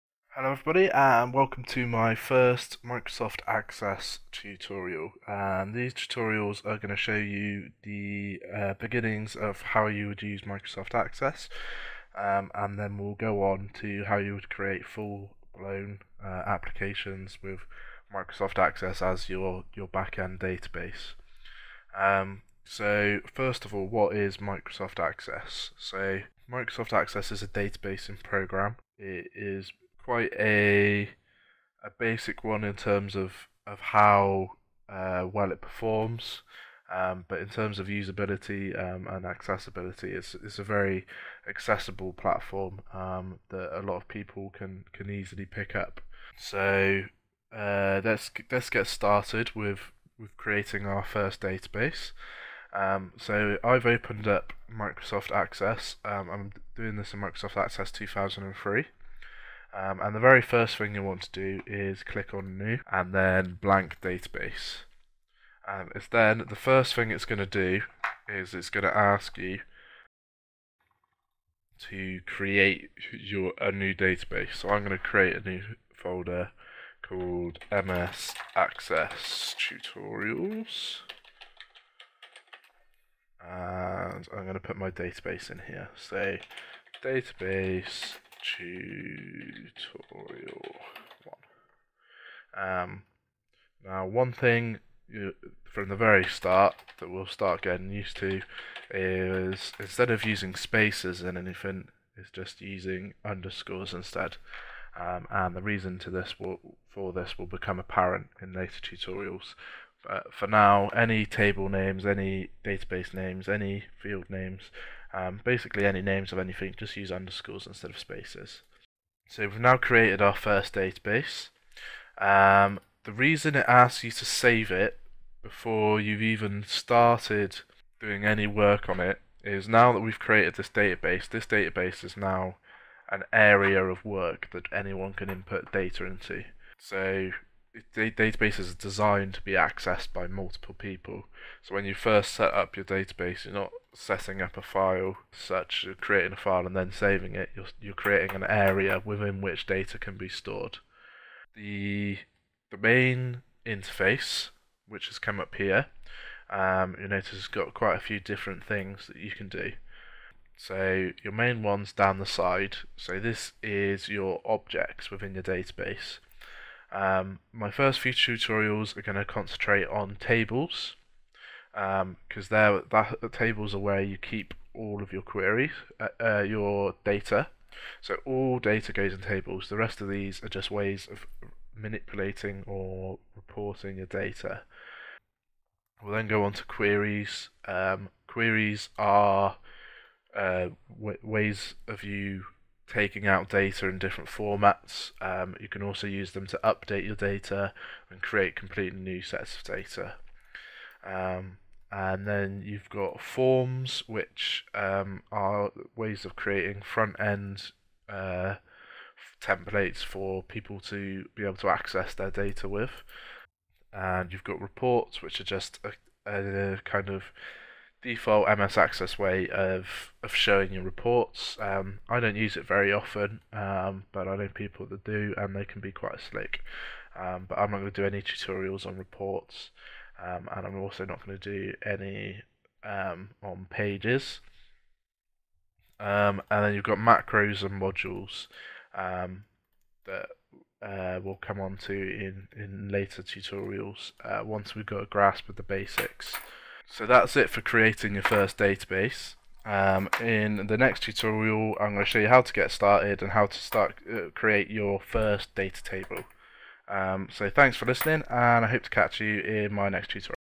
Microsoft Access Basics Tutorial 1 - Creating your first Database
How to create your first MS Access Database!